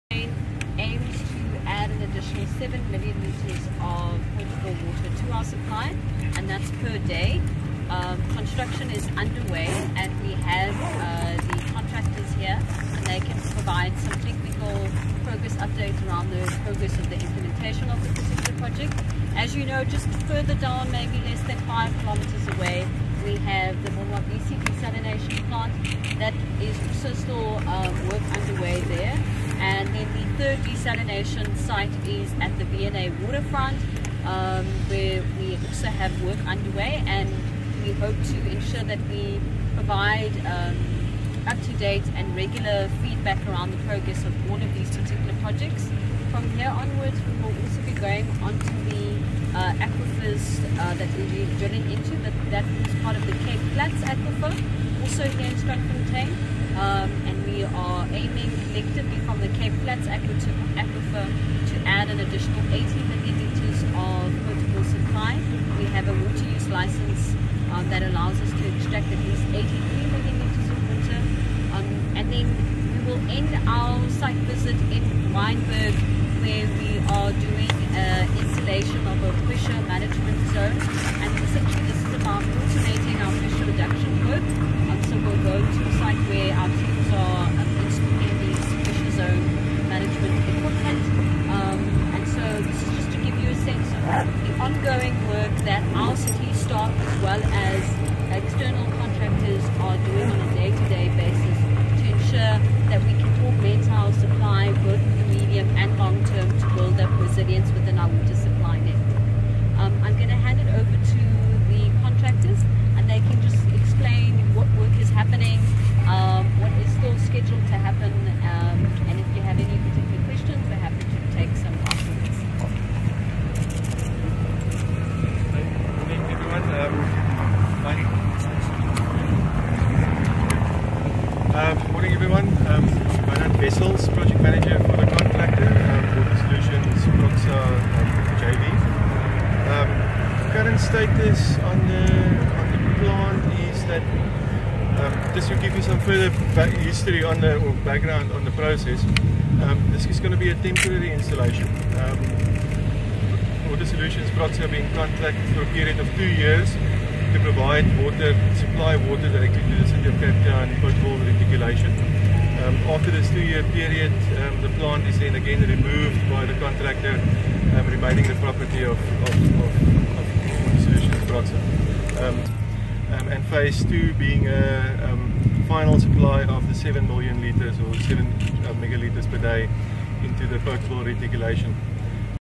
Cape Town pushes drilling and desalination to avoid Day Zero
Cape Town explores desalination and drilling of aquifers to get through the water crisis.